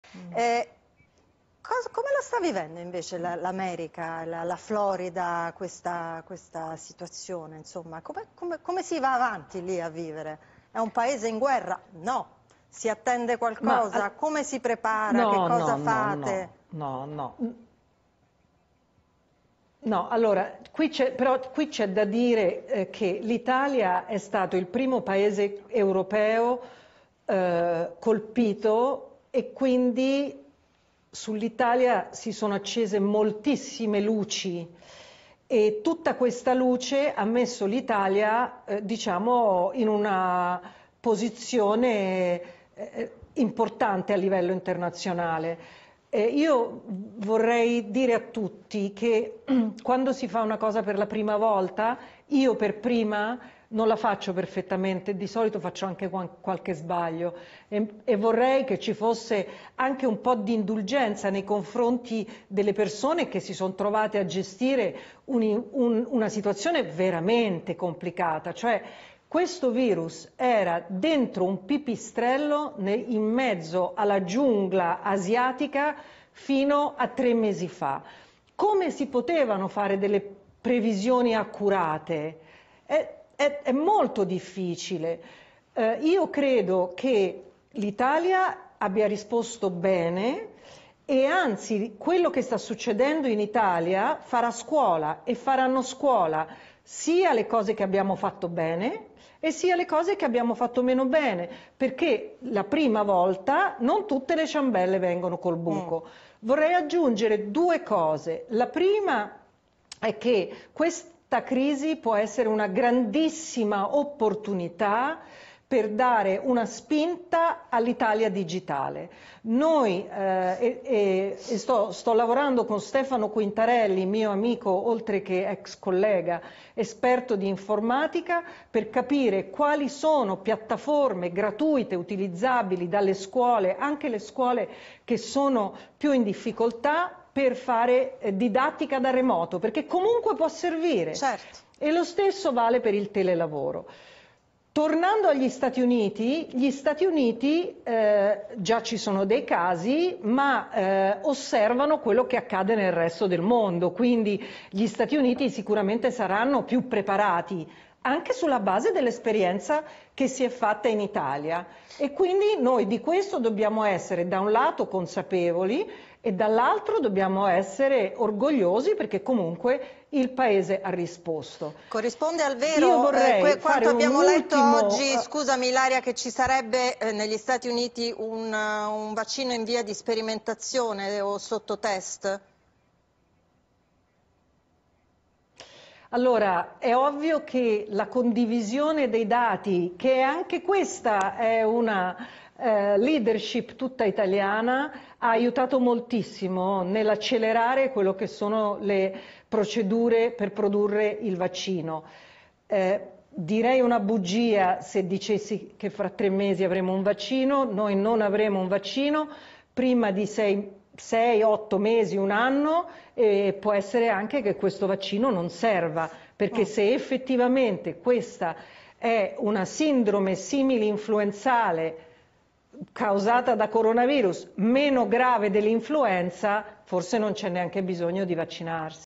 Coronavirus, la virologa Ilaria Capua: "Non avremo un vaccino prima di 6/8 mesi"
La virologa Ilaria Capua: "Non avremo un vaccino prima di 6/8 mesi e può essere che questo vaccino non serva"